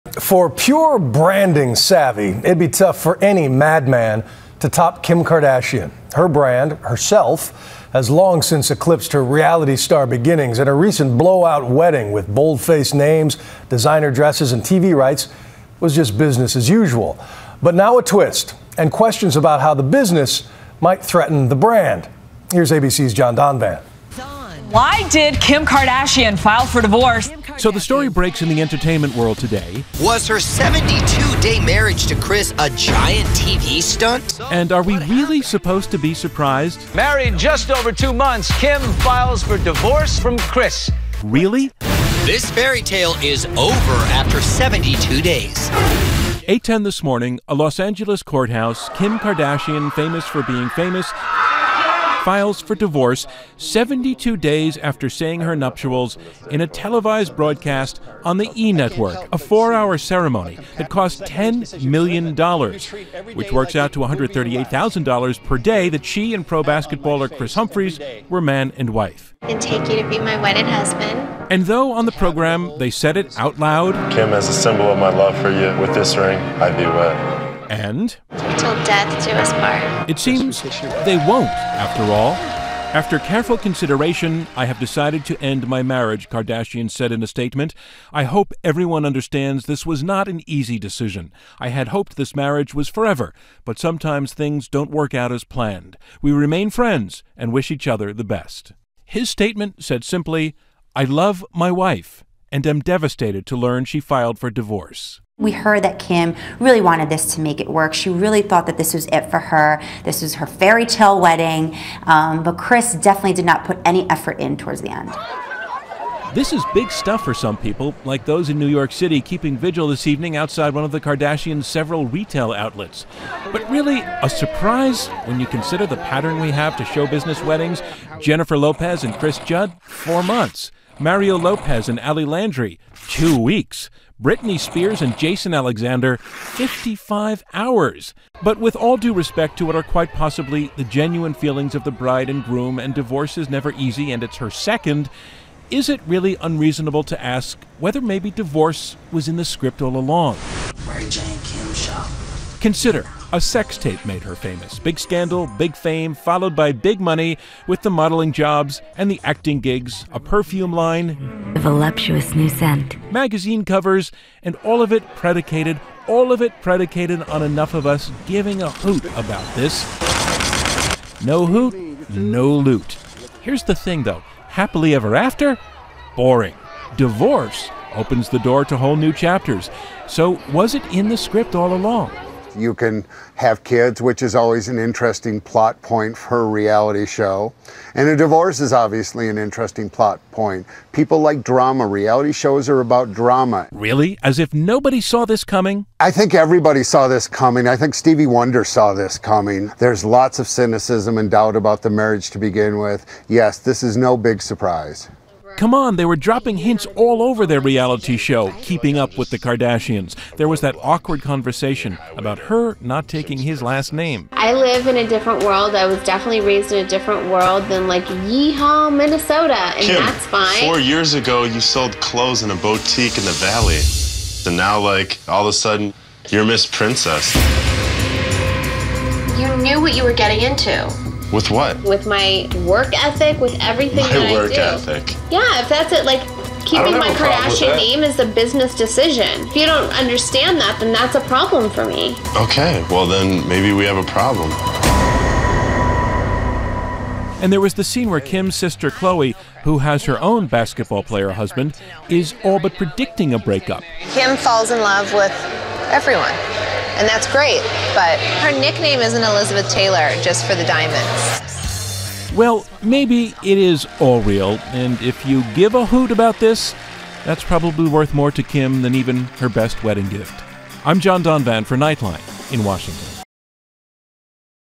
Was Kardashian Wedding a Stunt?
Kim Kardashian files for divorce 72 days after a multi-million-dollar wedding.